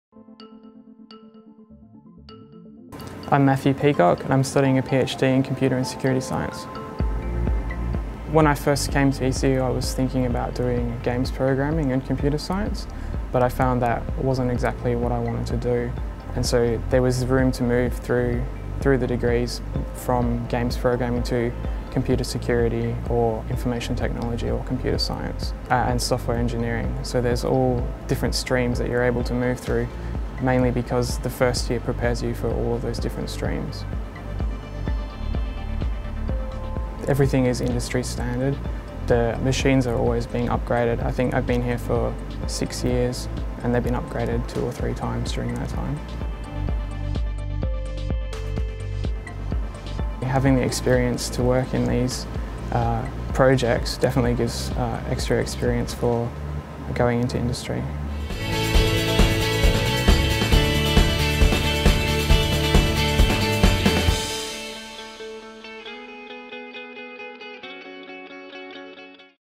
Why I'm Studying Computer and Security Science With ECU - Matthew's Story
Matthew Peacock, a student in the Doctor of Philosophy (PhD) in Computer and Security Science, talks about studying the course with Edith Cowan University (ECU). Matthew discusses how his initial plan to study Games Programming and Computer Science as an undergraduate changed. He discusses how, thanks to the course structure, he could move through degrees to study the discipline that he wanted to.